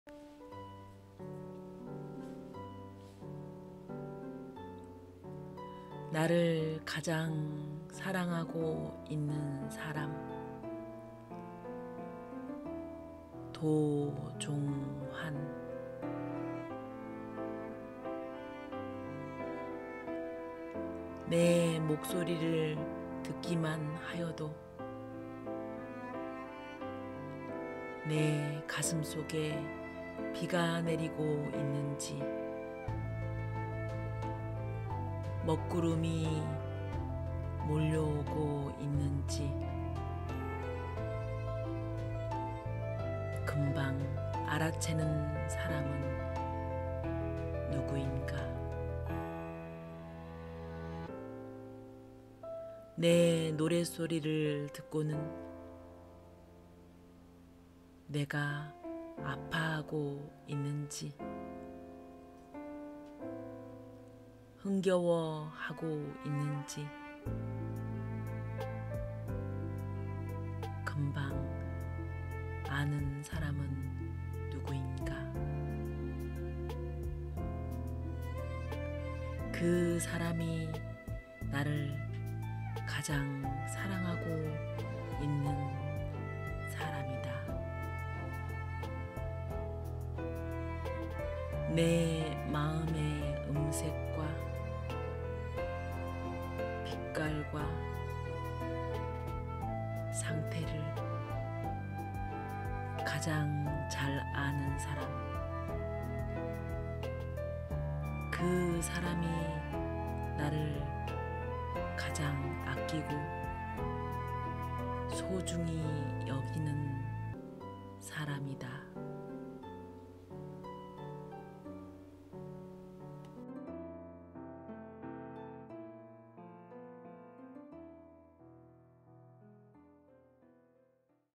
나를 가장 사랑하고 있는 사람 도종환 시인 시낭송 라벤더의 시가흐르는 풍경
추운 겨울 서로의 마음을 따뜻하게 감싸주는 잔잔한 감동시입니다😍